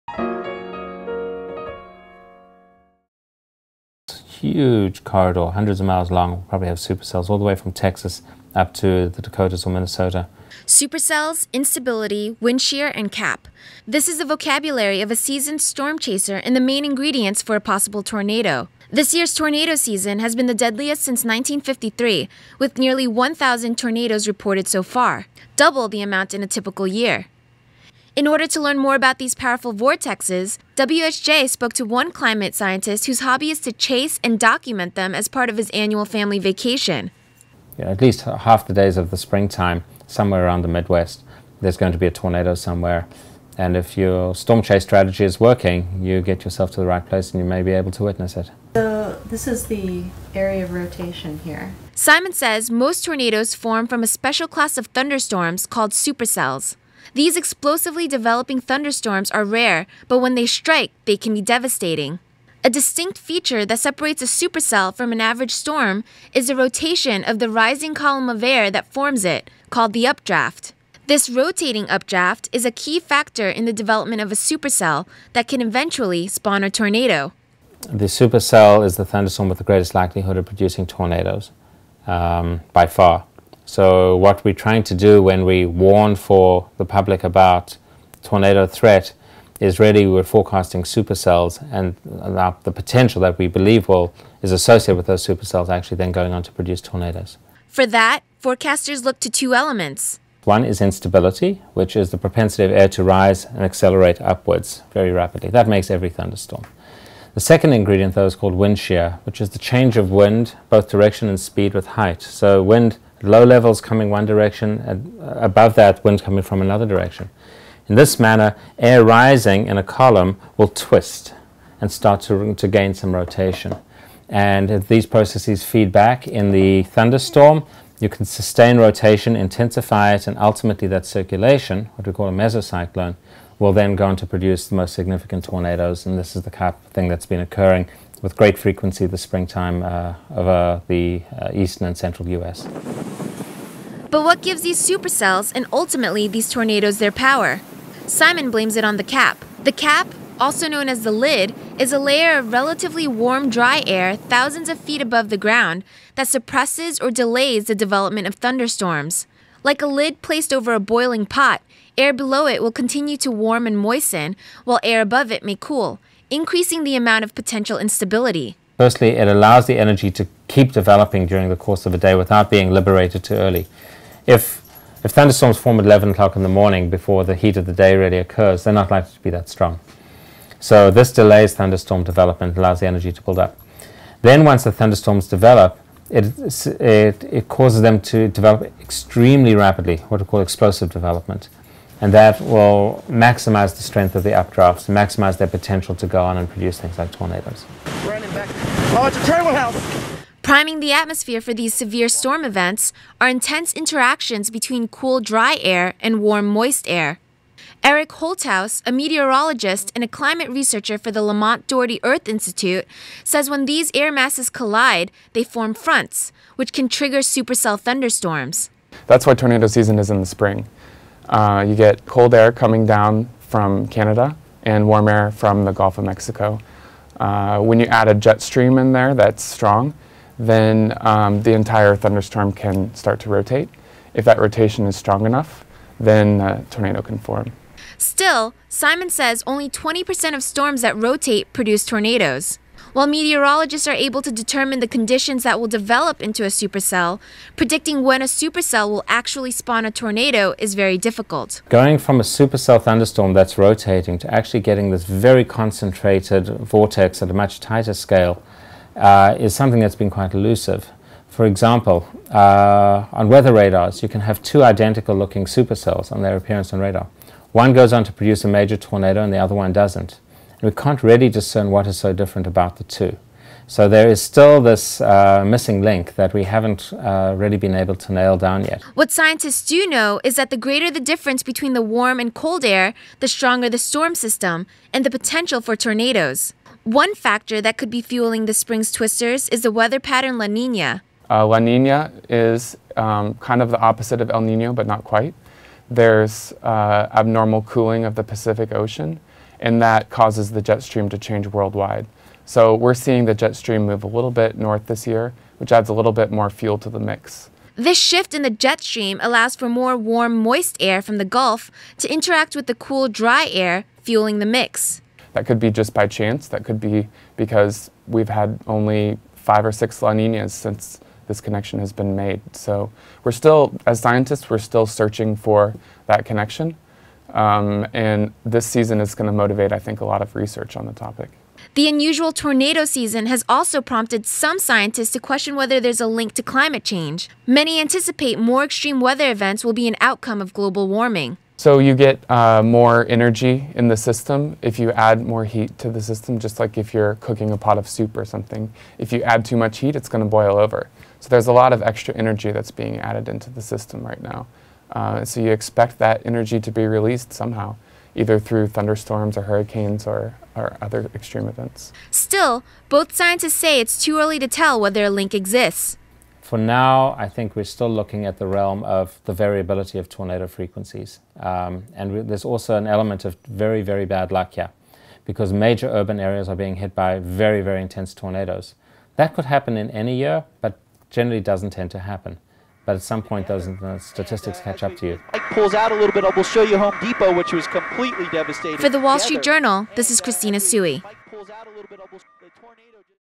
Tornadoes Explained, Storm Chaser Talks Science
Seasoned storm chaser and climate scientist Anton Seimon explains how tornadoes are formed. Plus, meteorologist Eric Holthaus discusses what's behind this year's deadly outbreak. WSJ's Christina Tsuei reports.